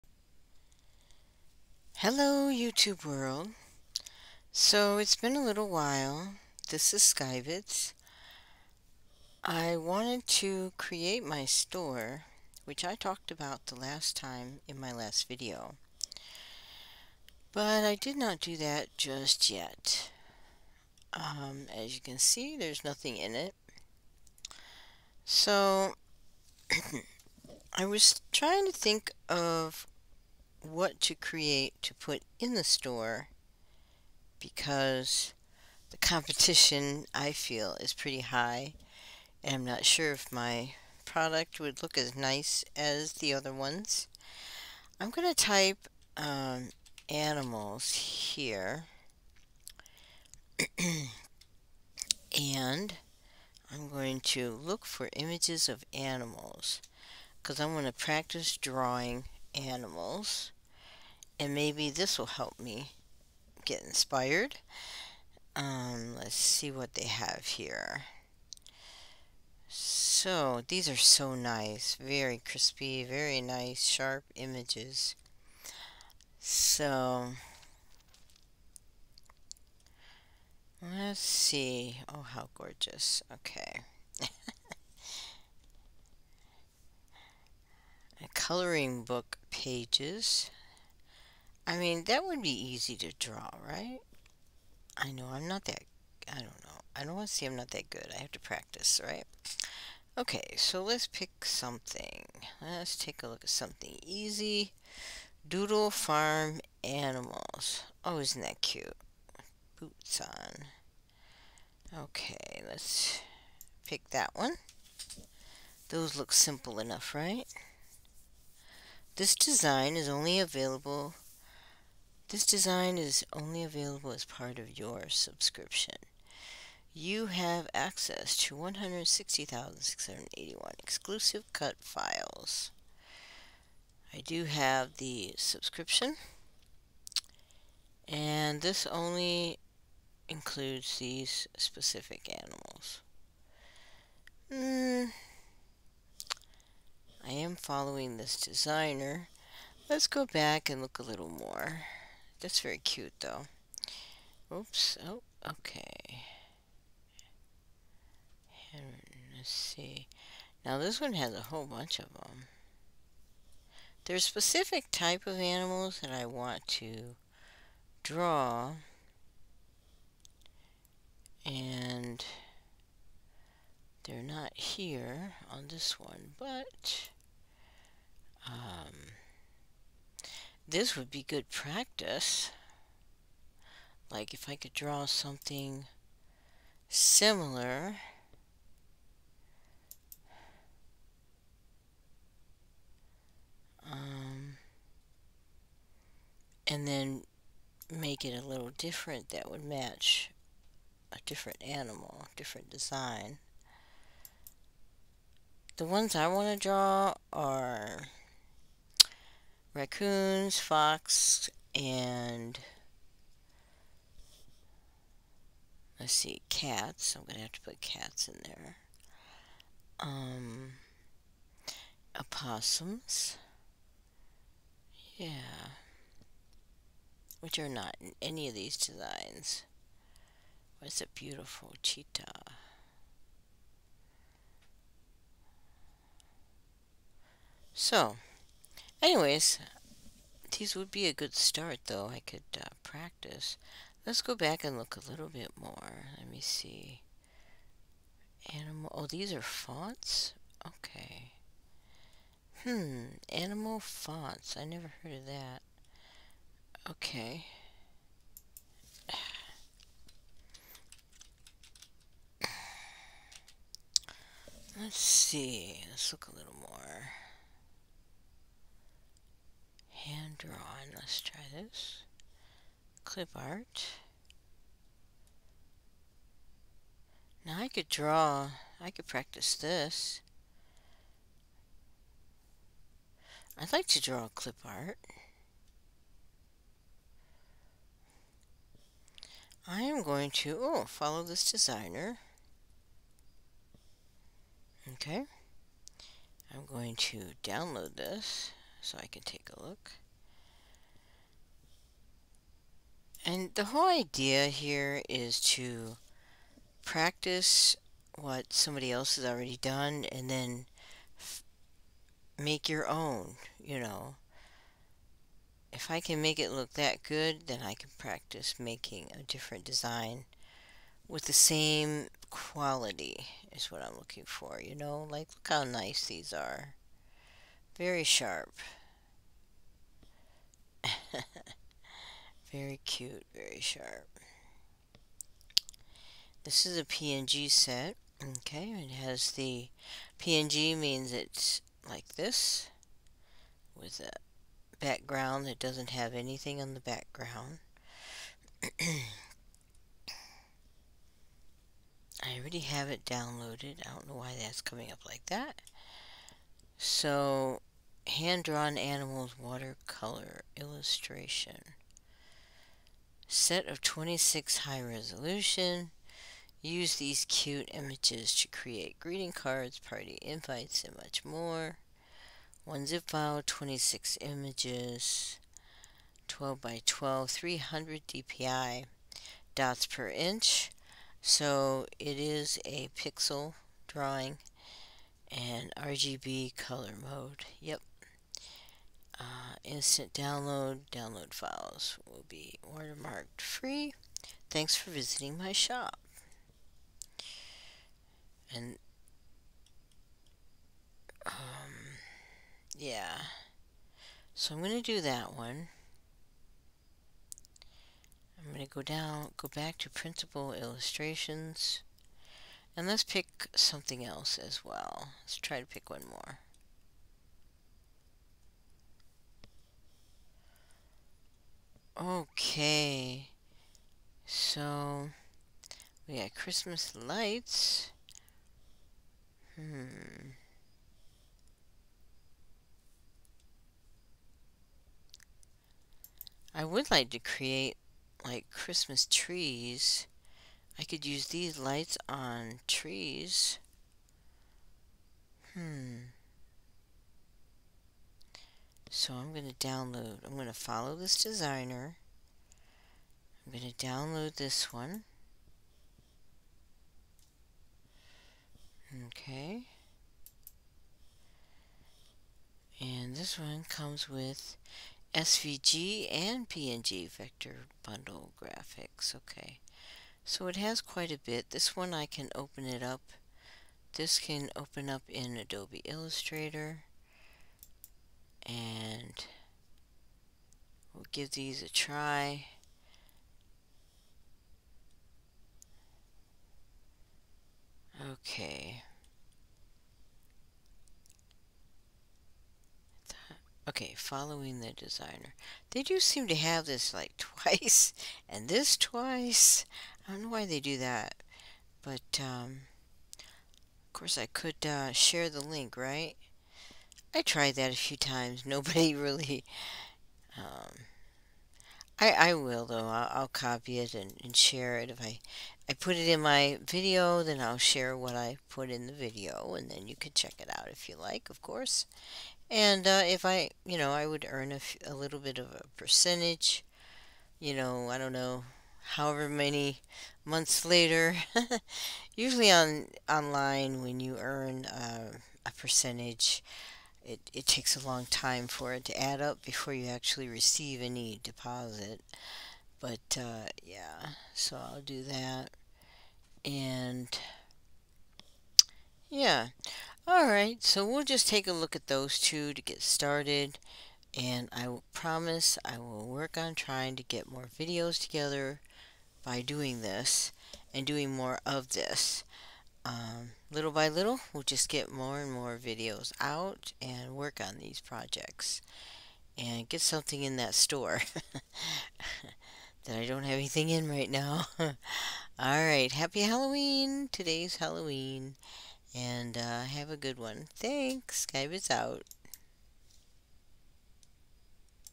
Checking Out More Stores in Creative Fabrica
Checking out a few stores on Creative Fabrica as I work on ideas for my store.

Hand Drawn Animals Clipart (PNG set) Graphic
by Blue Bell

Christmas Light SVG PNG Vector Bundle
by MySpaceGarden

About Rainbow Ribbon Christmas Trees Graphic
by Digital Curio

About Animals Font
by Minimalistartstudio

About Doodle Farm Animals Craft Design
by Creative Fabrica Crafts